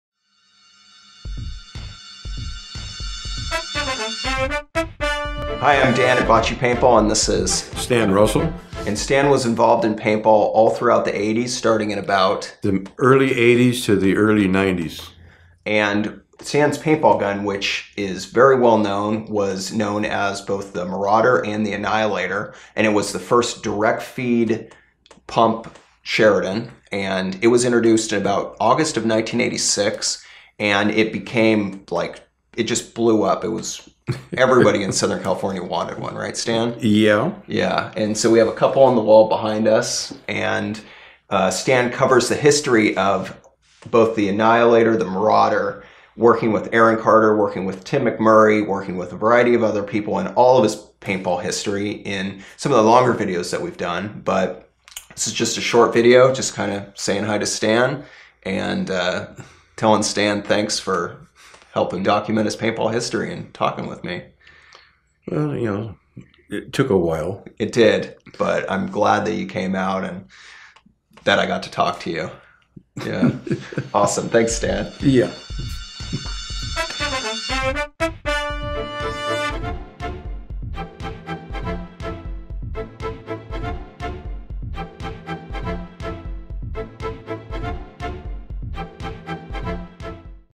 Stan Russell of South Bay Arms, Carter Machine, Mac 1 and Lapco
During my second meeting with Stanley Russell yesterday (2-20-16) we recorded a quick introduction. Stan's innovation and collaborations in paintball blew my mind. Here is a quick list of projects Stan was involved in:
Modifying Crosman pistols for .62 caliber (pre 3357)
Master Blaster
Modifying the Darkside Uzi
Sheridan Speed Feed
Dual 12 gram co2 adapter
Building what Jessica Sparks remembers as the first 12 gram quick changer
Delrin pump handle for the Sheridan
Lower Tube constant Air adapters
Annihilator (List Long Barrel Direct Feed Sheridan)
L stocks
T Stocks
Working with Earon Carter as South Bay Arms
Working with Tim McMurray
Working Colin Thompson
Building Hard Chromed Sheridans
Adding Top Rails to Sheridans
Building PVC bulk loaders
Building Dial a Welt (RVAs) for Sheridans
Doing Sheridan Valve Work.
Make Bottomline Tank Brackets
Working Around Colin Thompson as Colin developed the bottomline.
Supplying Steve "Gunner" Tetz with Annihilators.
Not selling to Bud Orr and as Bud has explained, causing Bud to build his own paintgun, the Sniper (which turned into the Autococker).
No one can deny that is an impressive list and I'm sure I'm forgetting details and Stan goes into details on nearly all of these subjects in the other videos we've recorded.